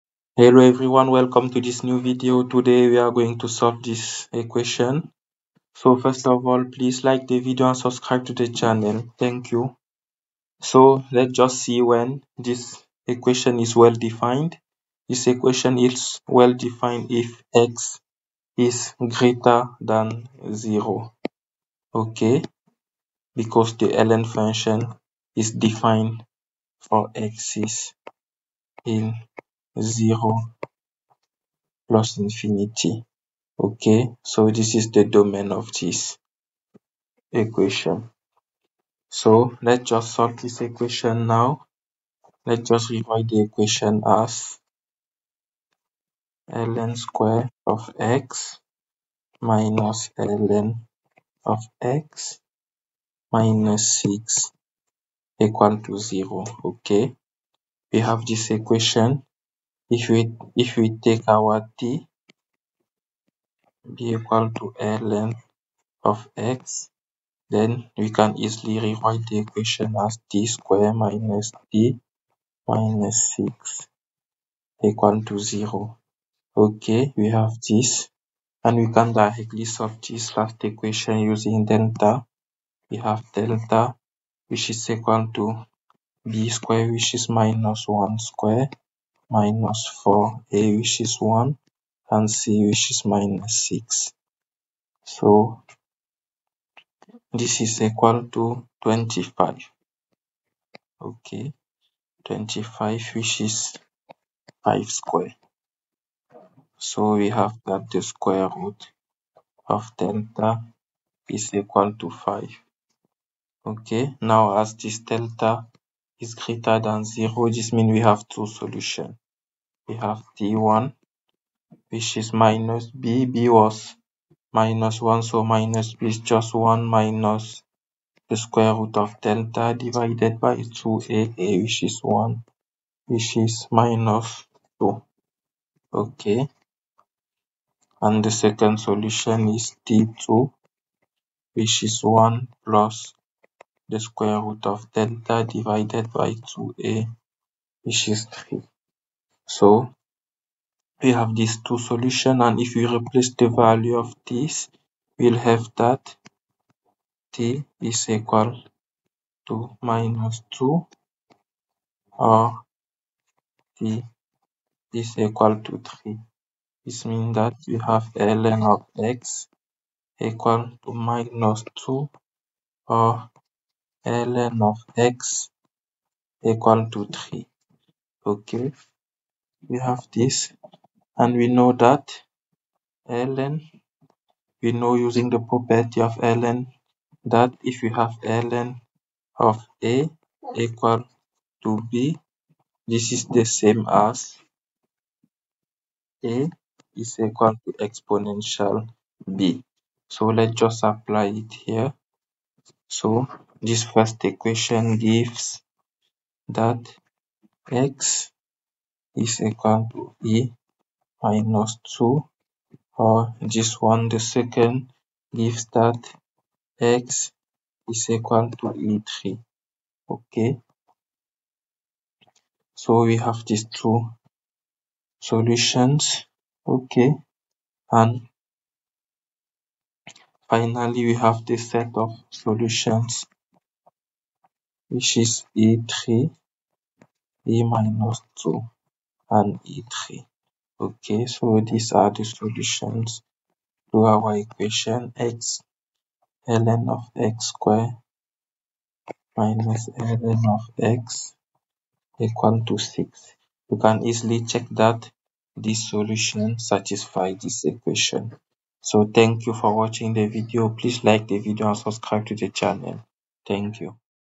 A nice logarithmic equation
We solve a nice logaritmic equation.
We show how to solve a nice logarithmic equation